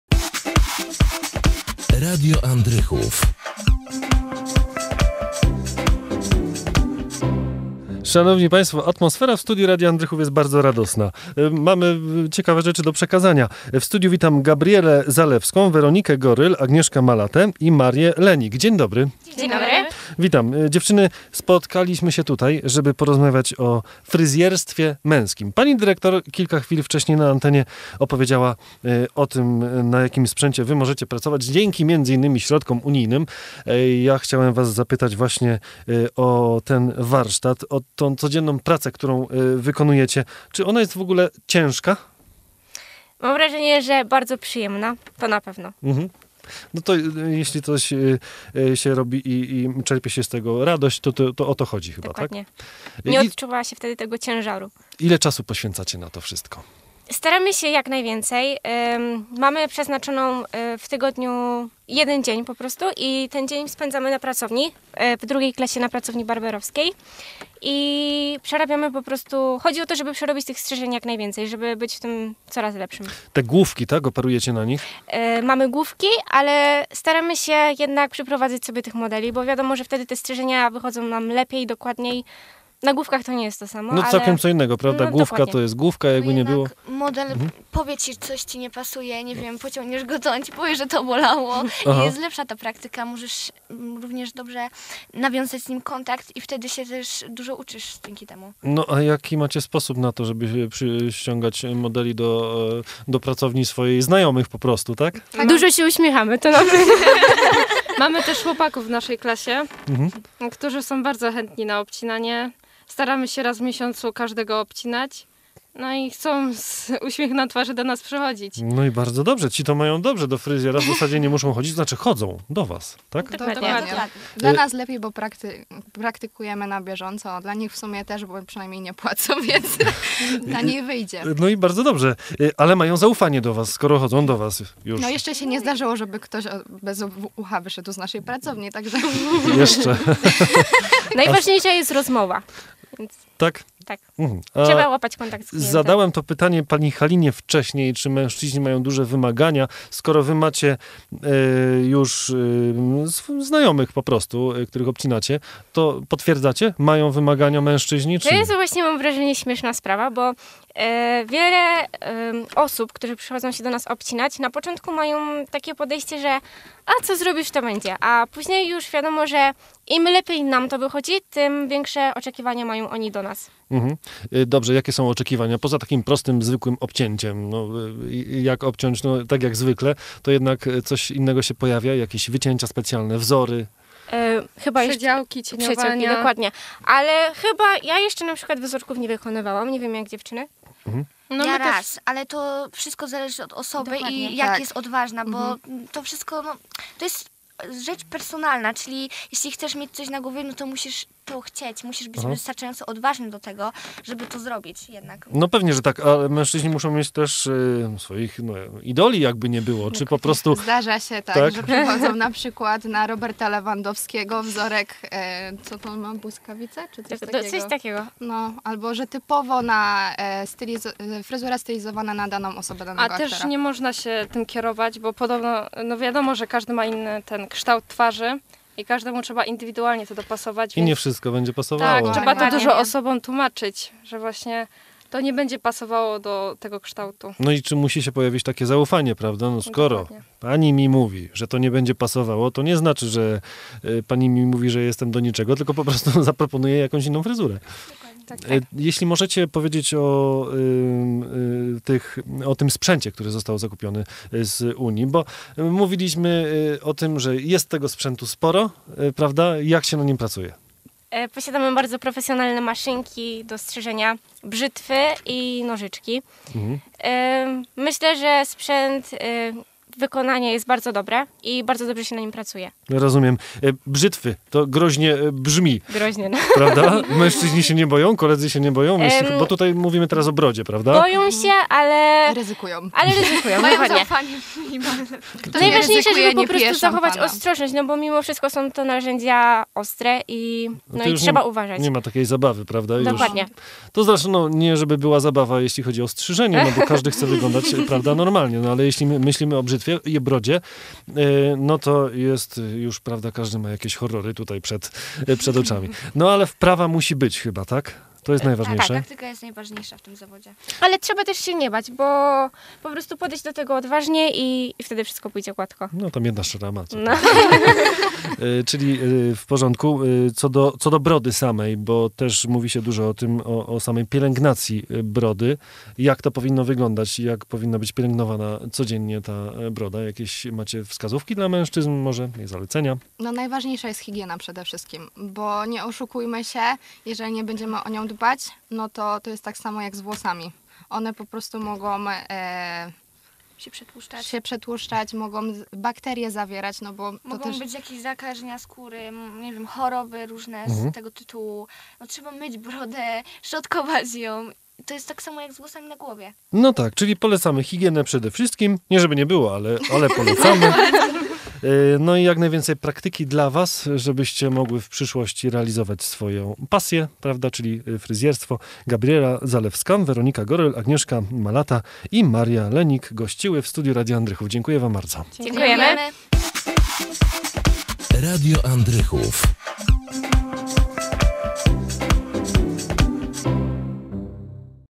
Fryzjerstwo męskie - Radio Andrychów - Barber-ki z Wadowic
Czym jest barberstwo? Czym różni się od zwykłego fryzjerstwa?
Gośćmi porannej audycji Robert Frasia w Radiu Andrychów były:  Maria Lenik, Agnieszka Malata, Weronika Goryl, Gabriela Zalewska z Technikum Usług Fryzjerskich w Wadowicach.

Rozmowa z 13 marca 2019 r.